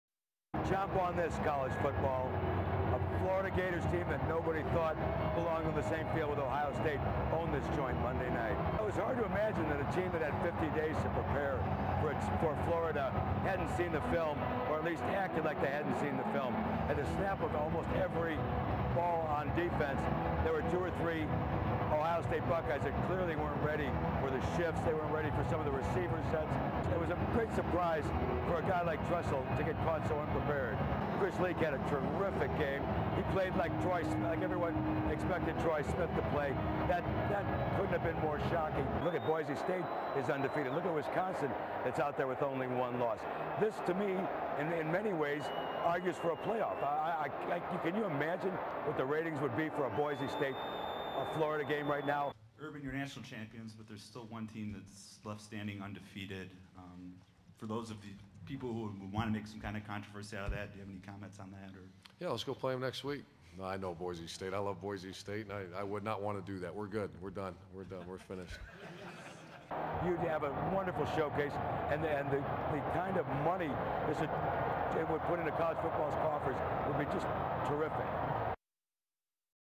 Litke: Ohio State’s Tressel caught unprepared for BCS title game.
(9 Jan 2007) HEADLINE: AP's Jim Litke on BCS Championship Game (fbc-jim-litke)
CAPTION: Litke: Ohio State's Tressel caught unprepared for BCS title game. (Jan. 9)

There is no unvoiced version available. This is an Associated Press columnist's comments on the game. It is A-P material, no restrictions.